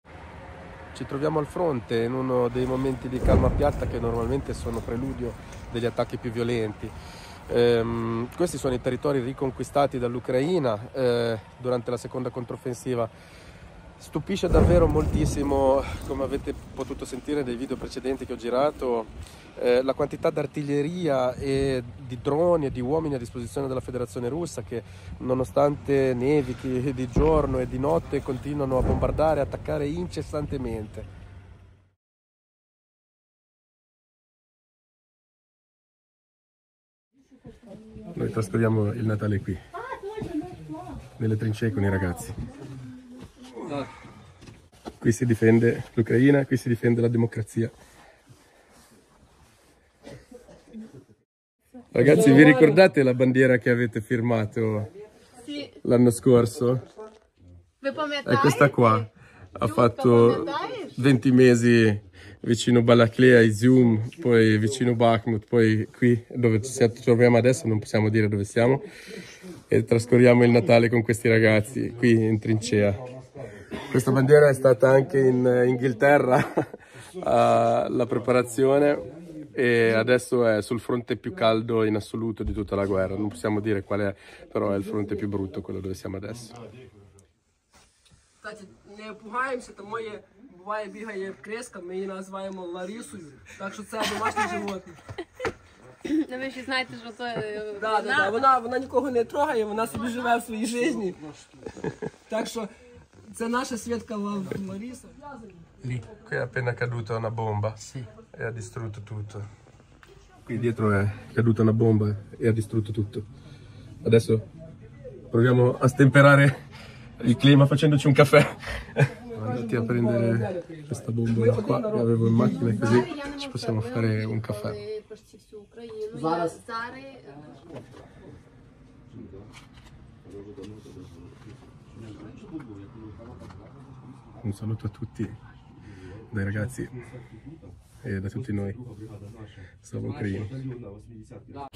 Ucraina, il nostro inviato dal fronte Sud
Iniziando dal fronte Sud - per ragioni di sicurezza non specifichiamo il luogo esatto in cui ci troviamo, limitandoci a descrivere ciò che si trova oltre le “linee Surovikin” sfondate dalle Forze armate ucraine nel corso della seconda controffensiva - dove la bandiera firmata l’anno scorso dai direttori di questo giornale e molti ragazzi delle comunità ucraine di Milano per sostenere tutti i soldati al fronte, è rimasta miracolosamente indenne e forte.

Servizio a cura di Giorgio Provinciali